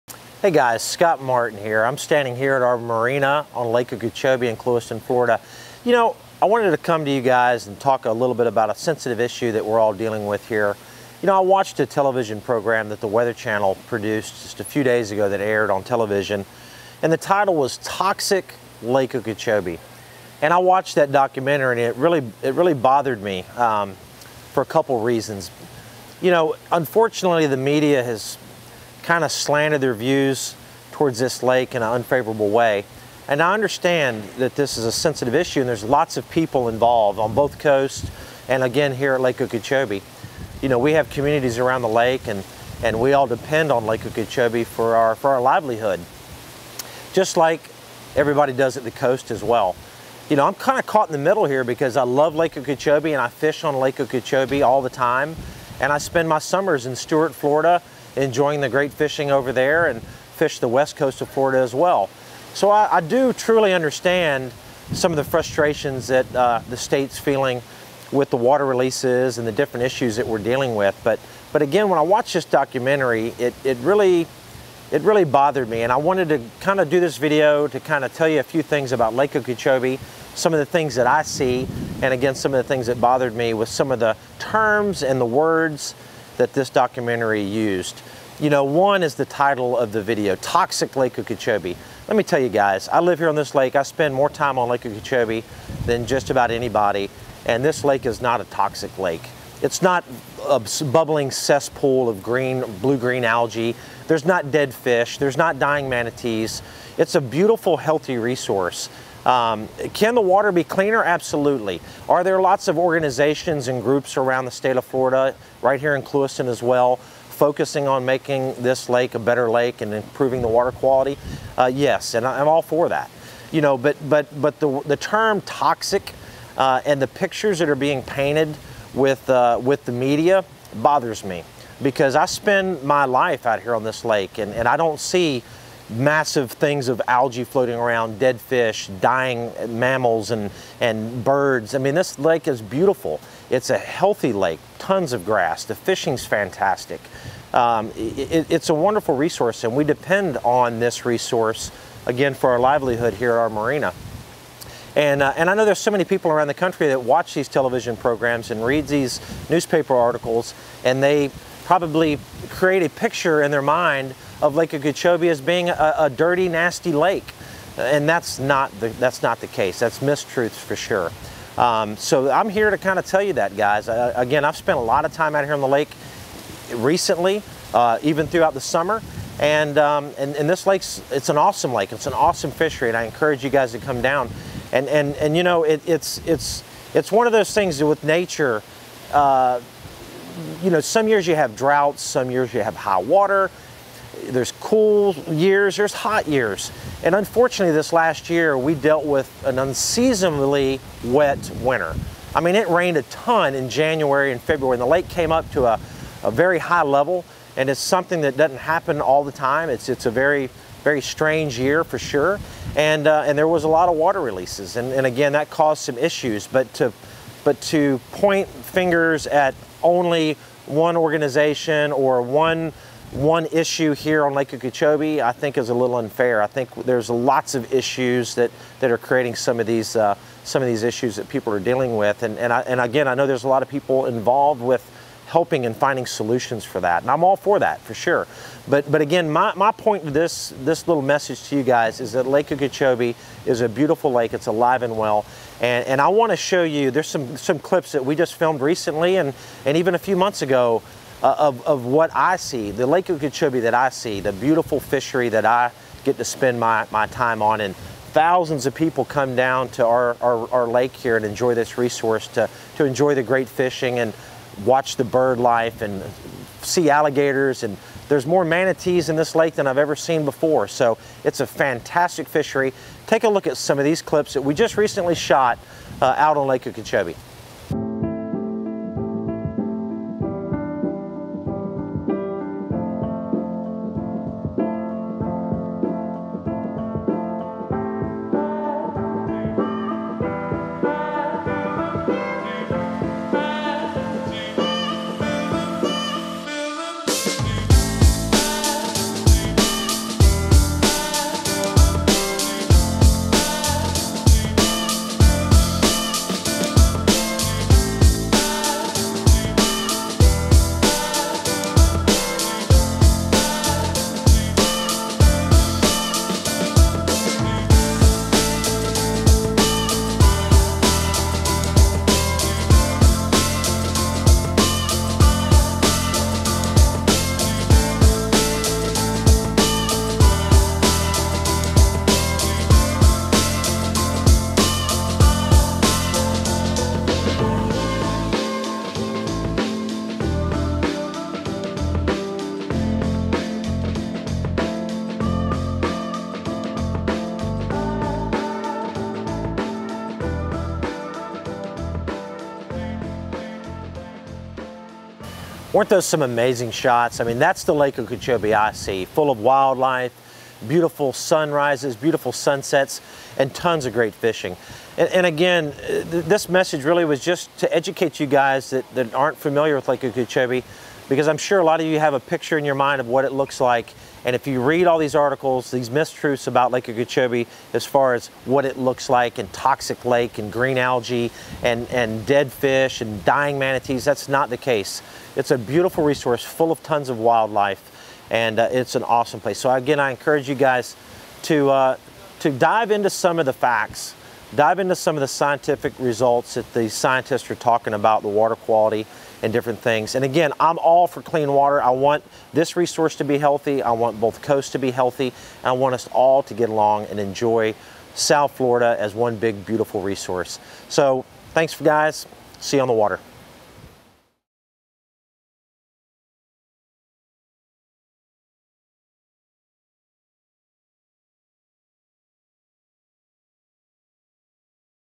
What I really think about Lake Okeechobee - Not a Toxic Cesspool
Lake Okeechobee has been in the media a lot this year about the water quality, algae and releases in South Florida. Hopefully this video will help you see the Lake as I see most everyday. Lake Okeechobee is one of the most beautiful lakes in the world and is full of fish, Alligators, Birds and Manatees.